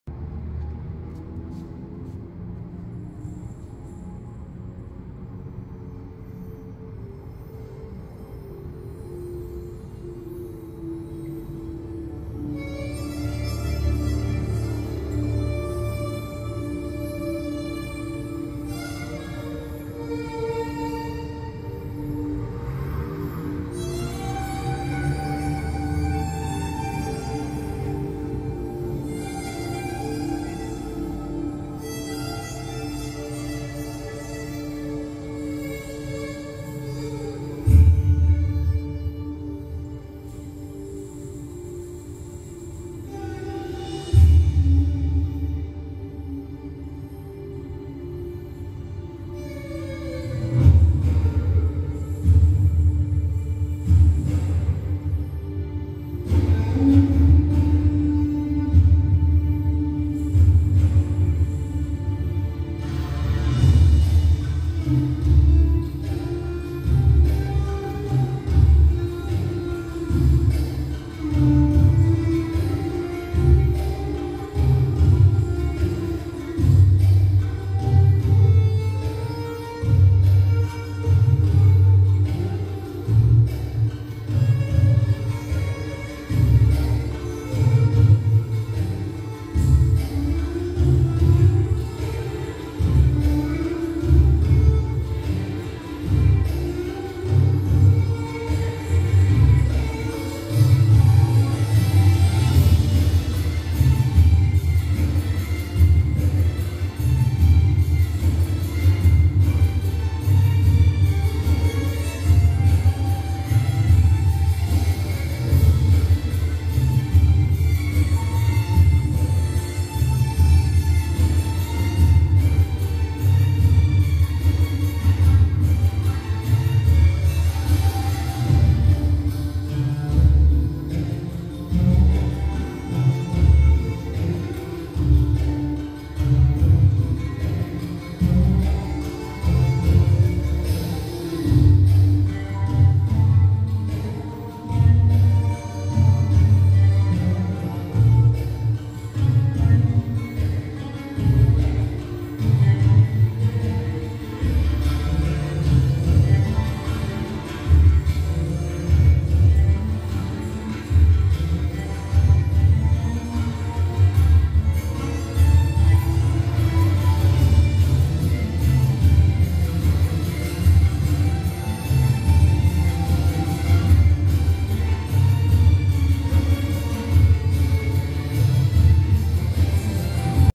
Belly dance - The jilted lover
This oriental style belly dance was choreographed by instructor Gittanjali from Sutra Studio, Klang, Malaysia & entitled 'The Jilted Lover'. The soulful movements were in tandem with the longing and pangs of a person who was abandoned and rejected. There were so many emotions and expressions to this routine and at times I found challenging. We do hope to have inspired you in your own belly dance journey. Enjoy!